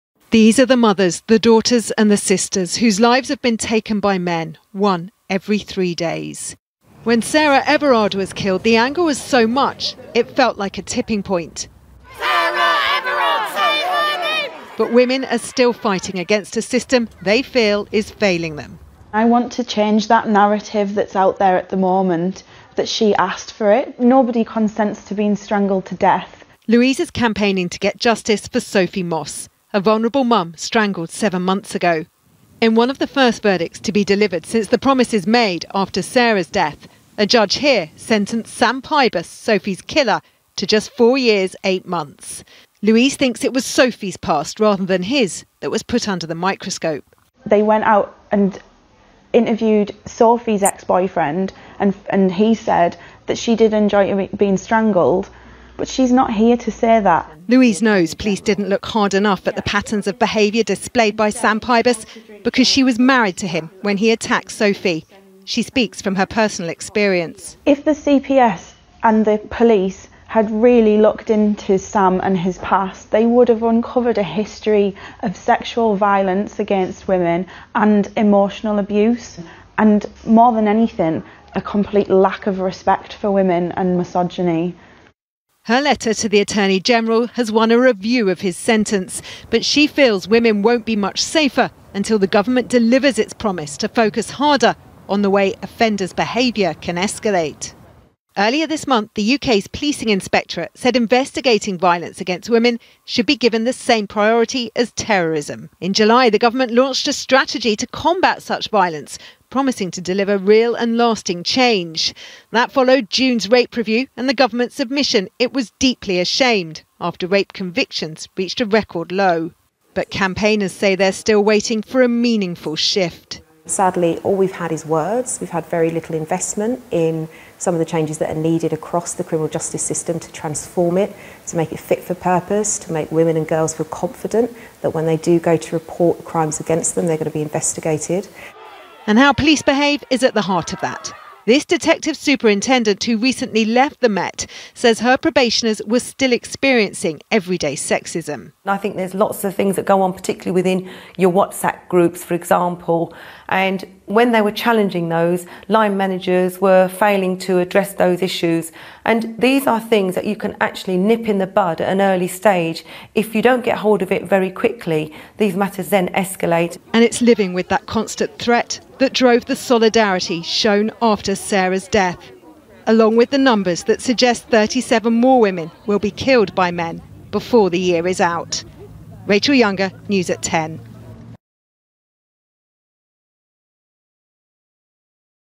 Have promises to end violence against women since Sarah Everard's murder had any effect?| ITV News
The Metropolitan Police says it is going to make changes to how it deals with violence against women and girls, since Sarah Everard's murder.

The government made promises to do more to protect them across the country.

But in that time, at least 78 other women have been killed by men.

So, ITV News has been trying to gauge whether those government promises have made any difference.